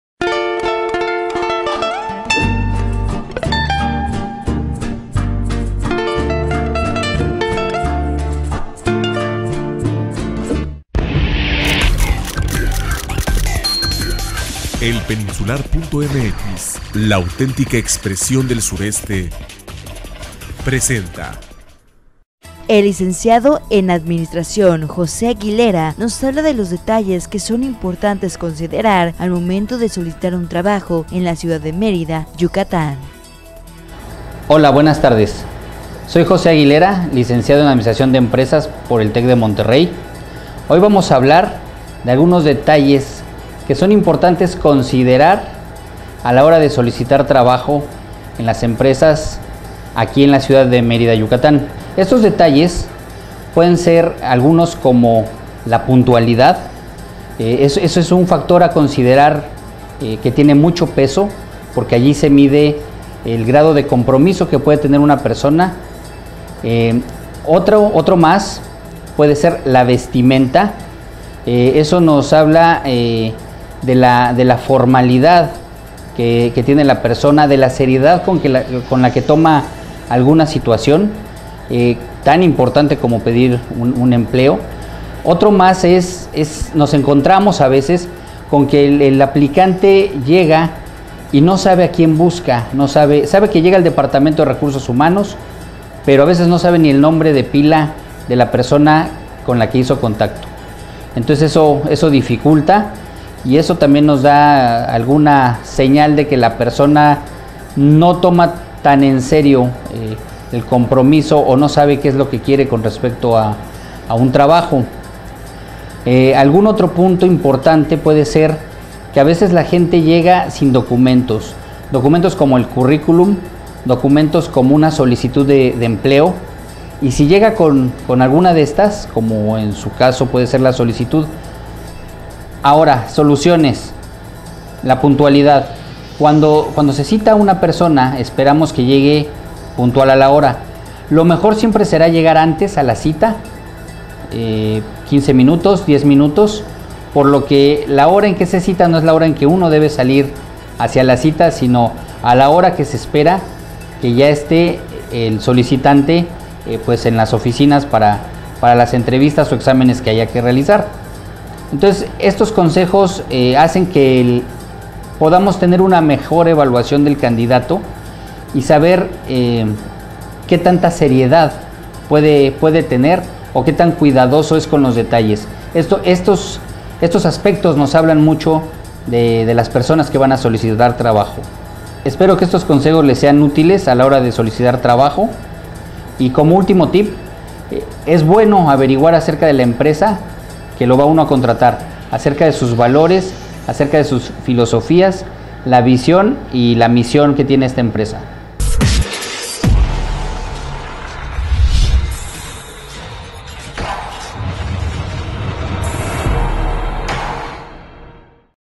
Puntos a considerar a la hora de acudir a una entrevista de trabajo
El Lic. en Administración  de empresas José Aguilera nos habla de los detalles que son importantes considerar al momento de solicitar un trabajo en la ciudad de Mérida Yucatán, debido a que mayormente de los reclutadores provienen de las zonas centro y norte del país.

Estos detalles sirven para saber identificar que actitudes son las correctas a la hora de presentarse en una entrevista de trabajo.

La puntualidad, la imagen y la  investigación que averiguas sobre la impresa ante de acudir a una entrevista de trabajo  son parte de las características más comunes que los reclutadores se fijan en la entrevista de trabajo.

Recuerden llegar con sus documentos, como una solicitud de empleo o su Curriculum Vitae.

Estos consejos sirven  para que los reclutadores puedan tener una mejor evaluación del candidato y saber que tanta seriedad  puede tener con esos detalles.